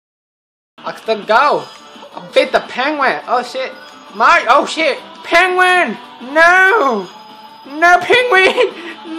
The Penguin
That moment when you think you're beating the penguin until he flashes by you and you struggling to keep up